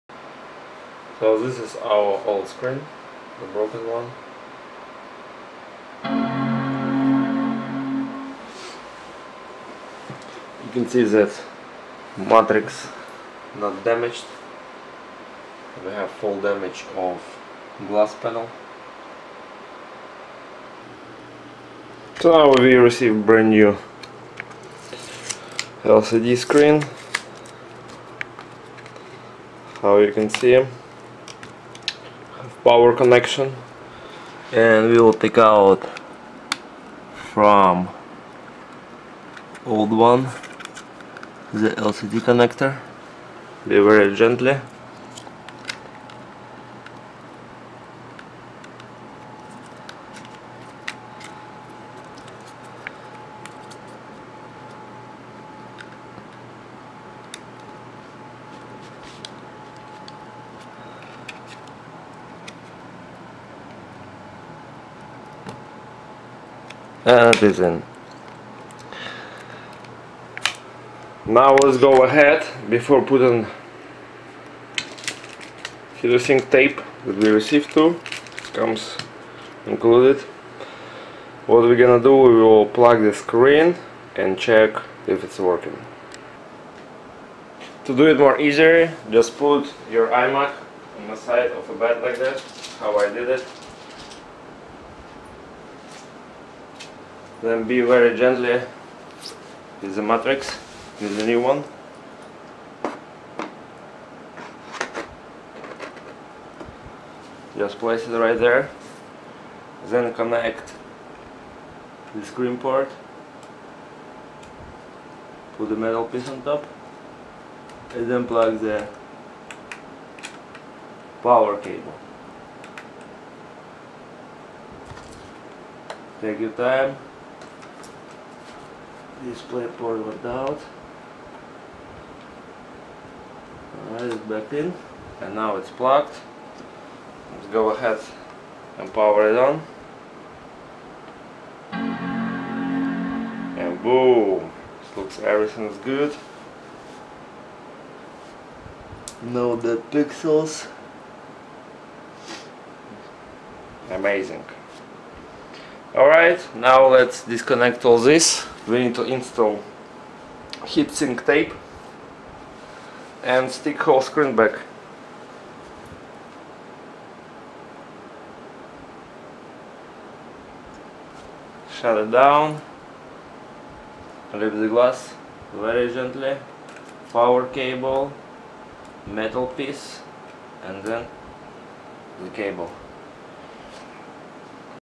Apple Imac - Screen installation A1418
Swipe Lcd connector for old screen to new, be careful wen you plug power cable with LCD connector A1418